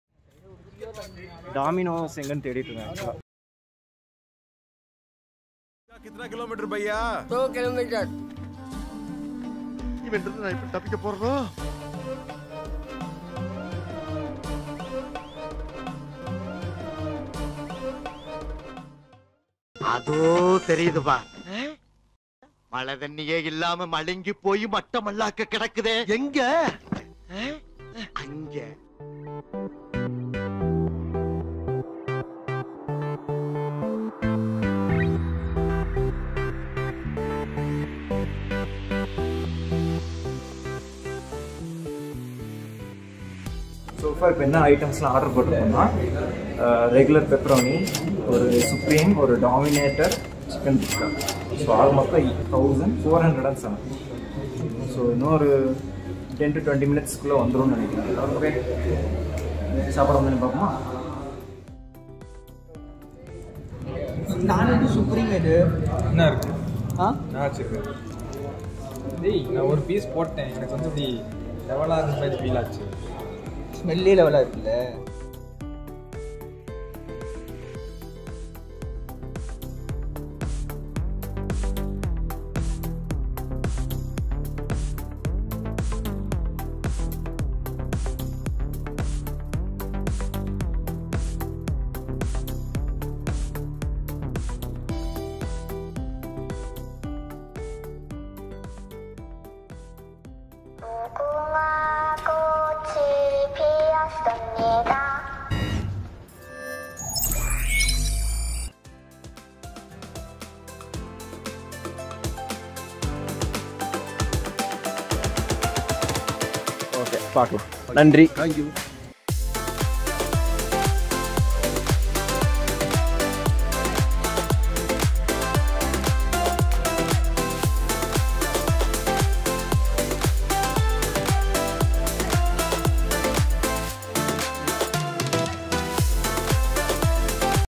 Kolapasi la kodaikanal Dominos lunch | Vlog(3/5)
Hey guys! We finally made it to kodaikanal dominos and we're really hungry..
Lets chow down on some pizzas!